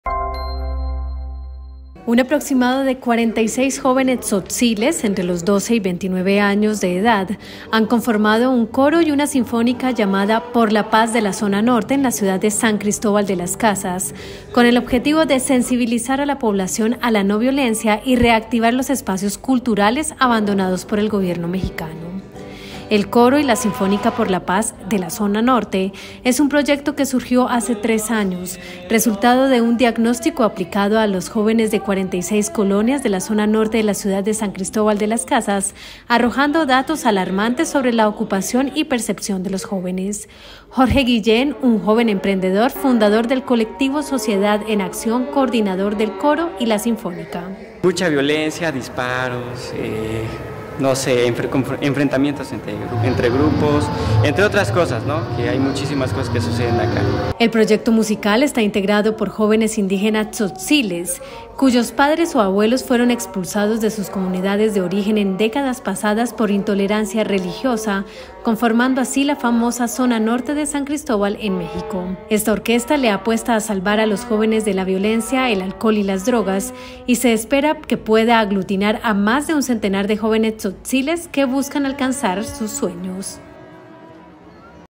Jóvenes indígenas integran orquesta para romper la violencia en el sur de Mexico
En una de las zonas más conflictivas del sureste mexicano, jóvenes indígenas tzotziles con padres desplazados de sus comunidades, se han unido en una orquesta y un coro para romper la violencia.

¡Visítenos!